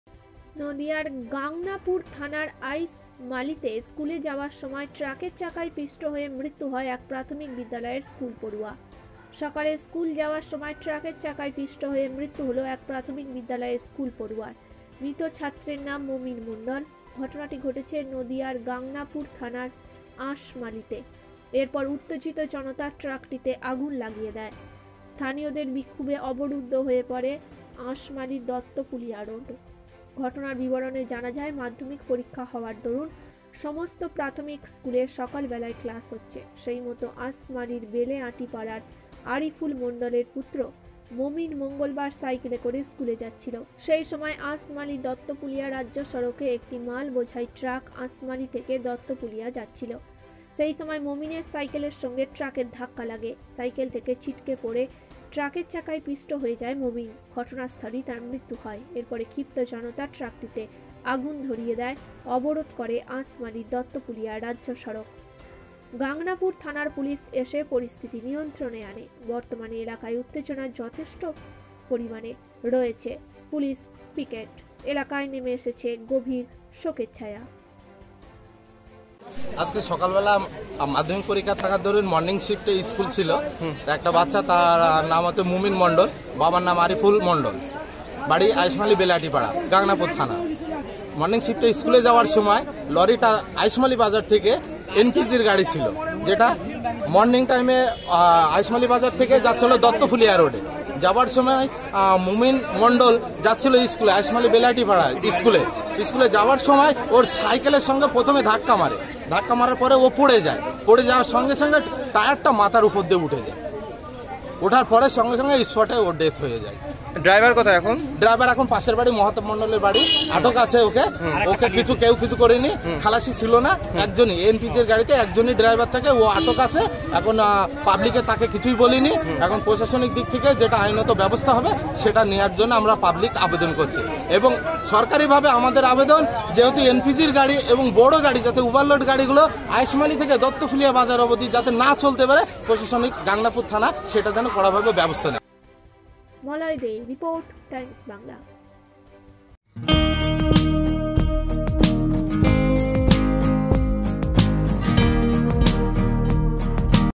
স্কুলে যাওয়ার সময় ট্রাকের চাকায় পিষ্ট হয়ে মৃত্যু হলো এক প্রাথমিক বিদ্যালয়ের স্কুল পড়ুয়া
নদীয়ার গাংনাপুর থানার আঁইশমালিতে স্কুলে যাওয়ার সময় ট্রাকের চাকায় পিষ্ট হয়ে মৃত্যু হলো এক প্রাথমিক বিদ্যালয়ের স্কুল পড়ুয়া